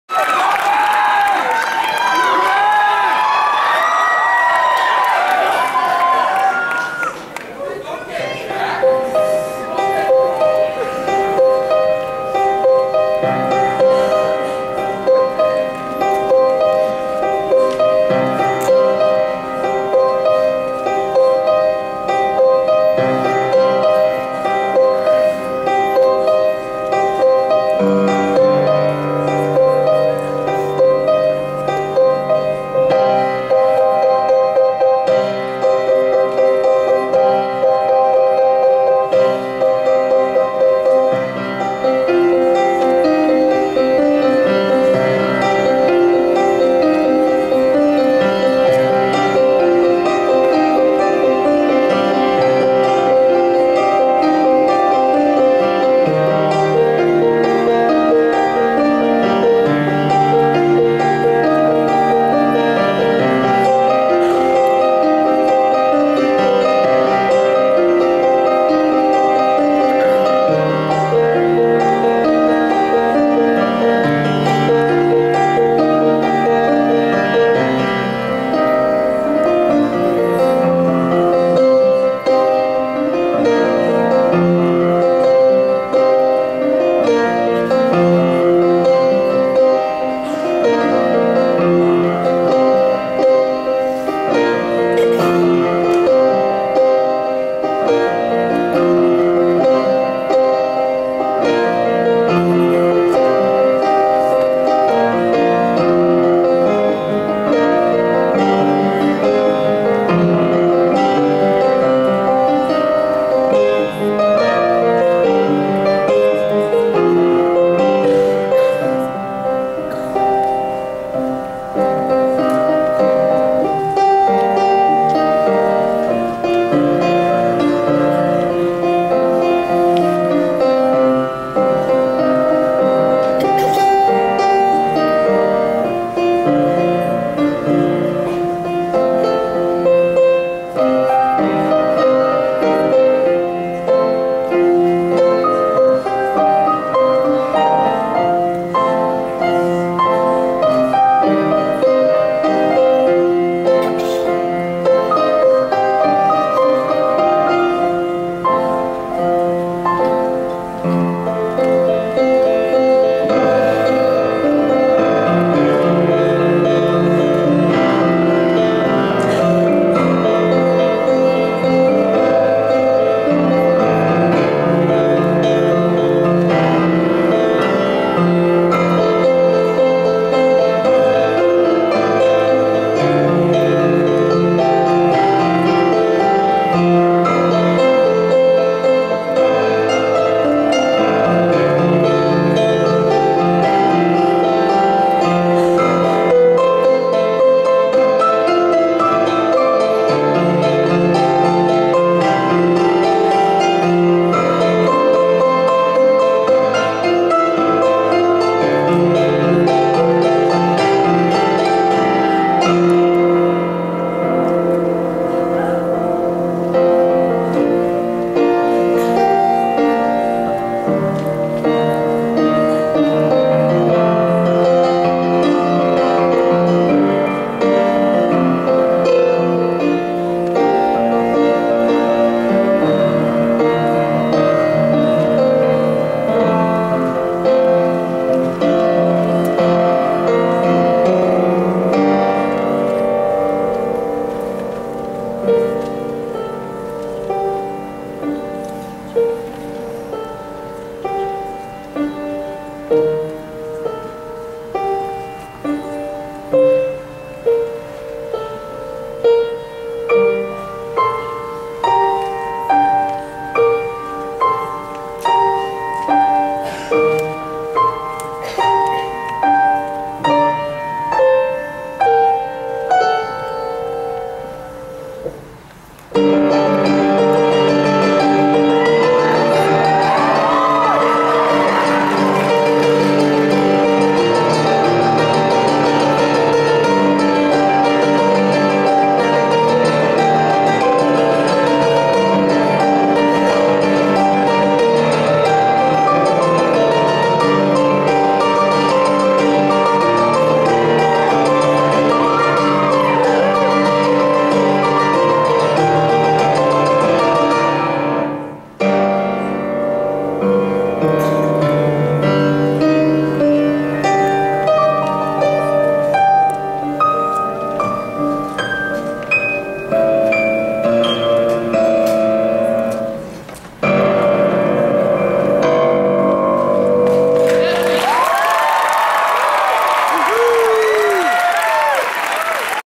Jack Robbins and Drew Miller 2011 Homestead AFS show
Dueling Pianos 2011 Homestead AFS show, Jack Robbins, Drew Miller